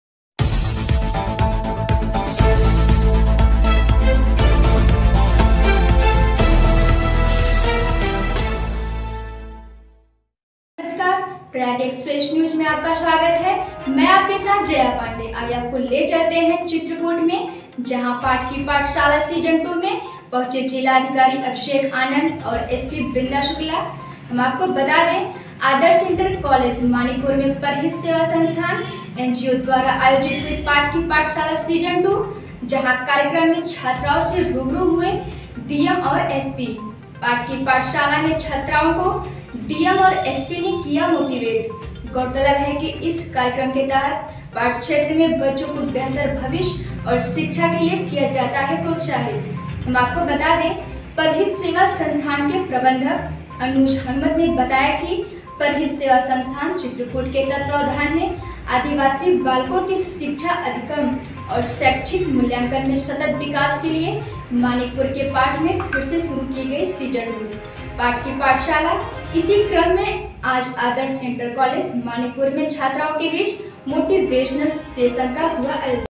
यूपी: पाठ की पाठशाला सीजन-02 में पहुँचे ज़िलाधिकारी अभिषेक आनंद और SP वृंदा शुक्ला..!
चित्रकूट ब्रेकिंग -

पाठा की पाठशाला सीजन-02 में पहुँचे ज़िलाधिकारी अभिषेक आनंद और एसपी वृंदा शुक्ला , आदर्श इंटर कॉलेज मानिकपुर में परहित सेवा संस्थान (NGO) द्वारा आयोजित हुई पाठा की पाठशाला सीजन 02, कार्यक्रम में छात्राओं से रूबरू हुए डीएम और एसपी, पाठा की पाठशाला में छात्राओं को डीएम और एसपी ने किया मोटिवेट , ग़ौरतलब हो कि इस कार्यक्रम के तहत पाठा क्षेत्र में बच्चों को बेहतर भविष्य और शिक्षा के लिए किया जाता है प्रोत्साहित , परहित सेवा संस्थान के प्रबंधक अनुज हनुमत ने बताया कीपरहित सेवा संस्थान चित्रकूट के तत्वाधान में आदिवासी बालकों के शिक्षा अधिगम और शैक्षिक मूल्यांकन में सतत विकास के लिए मानिकपुर के पाठा में फिर से शुरू की गई सीजन-2 'पाठा की पाठशाला , इसी क्रम में आज आदर्श इंटर कालेज मानिकपुर में छात्राओं के बीच मोटिवेशनल सेशन का हुआ आयोजन

संवाददाता
 बद्री विशाल
चित्रकूट उत्तर प्रदेश